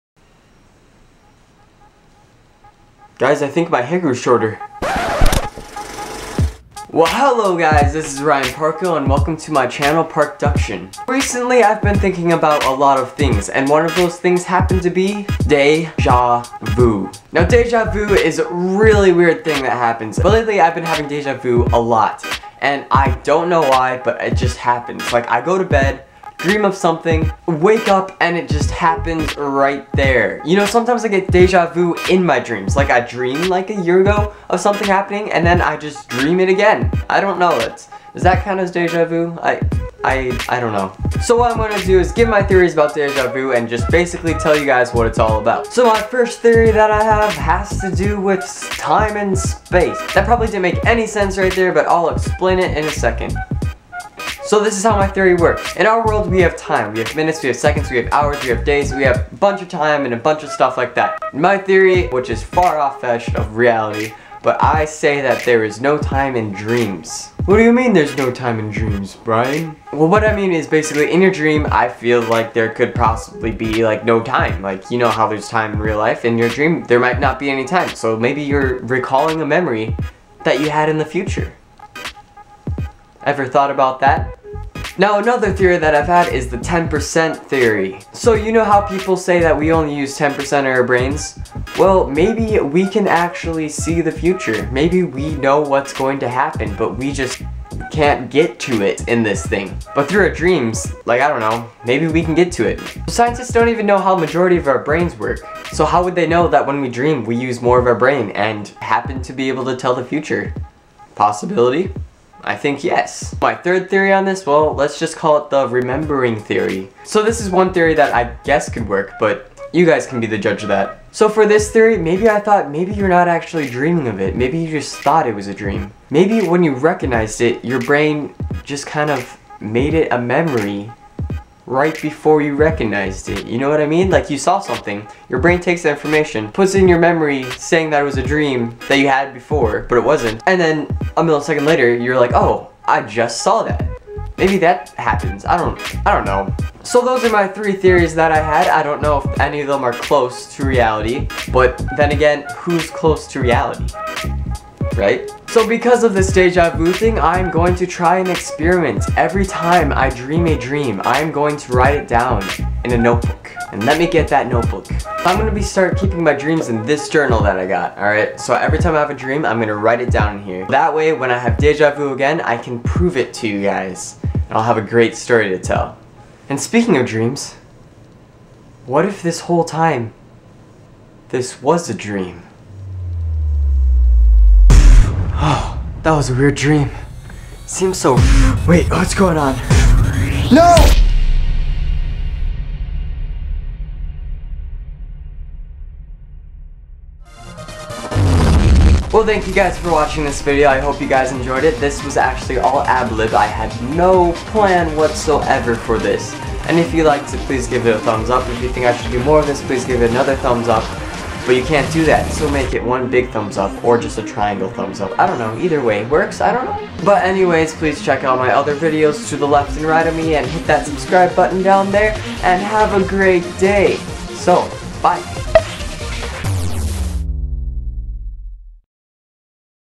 Why Does Deja Vu Happen? | ParcDuction
Have you ever wondered why deja vu happens? Well I certainly have. So this video is about my deja vu theories on how deja vu happens and what I think it's about. P.S Stay till the end of the video cause it's pretty awesome!

Outro Music help:
-Flick3r, Courtesy of Freesound
-DirtyJewbs, Courtesy of Freesound

If you just so Happened to SCROLL down this far in the Description then comment...
"I Just had Deja Vu! "

P.S. If you went even farther and came to this then you are the most awesomeness person ever! :)